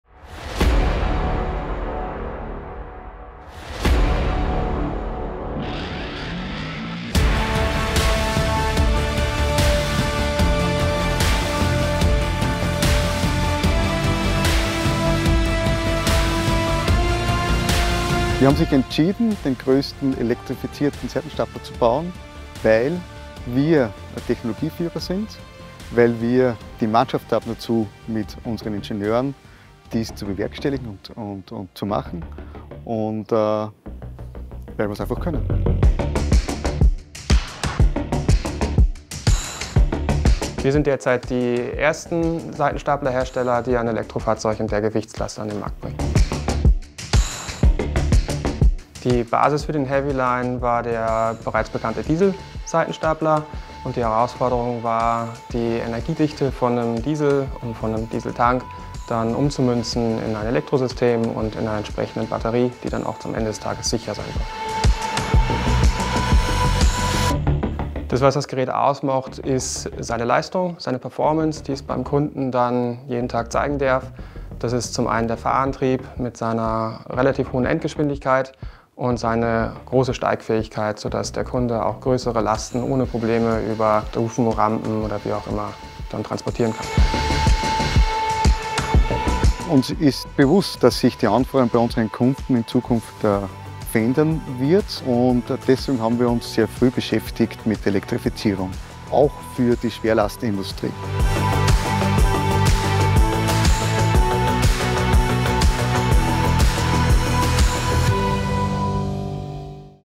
EQ 120 Elektro Schwerlaststapler heavyline powered by bulmor . Elektro Seitenstapler mit 12 T.
Den EQ 120 Heavy Line Elektro-Seitenstapler entwickelte Bulmor gemeinsam mit dem österreichischen Ingenieurholzbau-Unternehmen WIEHAG mit Sitz in Altheim und elektrifiziert somit den ersten Heavy Line der 12 Tonnen Klasse in der Branche.
„Wir haben mit dem E-Antrieb bereits lange Erfahrung und elektrifizieren diese als Einziger am Markt nun bis in die 12 Tonnen Klasse“, freut sich Verkaufsleiter Andreas Leonhartsberger.

Damit erhält das Unternehmen WIEHAG von BULMOR das Beste aus beiden Seitenstapler-Welten: eine noch stärkere Hubleistung als ein Dieselstapler und die ohne Emissionen, Betriebsgeräusche und Maschinenvibration.

Neben der Wirtschaftlichkeit ist für das Unternehmen die geringere Lärm- und Abgasbelastung ein wesentlicher Grund, um beim Neukauf eines Seitenstaplers auf Elektroantrieb umzusteigen. Der geräuscharme Elektroantrieb schont nicht nur Anrainer und Mitarbeiter, sondern auch das Transportgut. Dieses kann schonend, ohne Risiko der Verschmutzung durch Dieselruß, bewegt werden. Ein großer Mehrwert, nachdem der Holzmarkt verstärkt auf umweltschonende und saubere Fertigung setzt.

50% schnellere Hubperformance als ein Diesel-Seitenstapler

Die einzigartige Geschwindigkeit und Leistungsfähigkeit dieses Heavy Line baut auf die neue Generation der Bulmor Elektro-Serie auf und ist das Ergebnis zahlreicher Entwicklungsschritte, welche diese bei BULMOR durchlief. Der EQ 120 besitzt alle Vorteile, welche die neu entwickelte Elektro-Generation von BULMOR bietet:
Ein Setting neuester Motortechnologie und Leistungssteuerung, welches perfekt aufeinander abgestimmt. Damit erreicht ein BULMOR EQ 120 unbeladen eine Hubgeschwindigkeit von bis zu 32 m/s – mit 80 % Last immer noch eine Hubgeschwindigkeit von 0,26 m/s.
Diese erhöhte Performance spiegelt sich auch im Wert der Steigfähigkeit wider. Mit Last beträgt diese über 20 Prozent - ohne Last schafft die Maschine eine Steigfähigkeit von bis zu 30 %.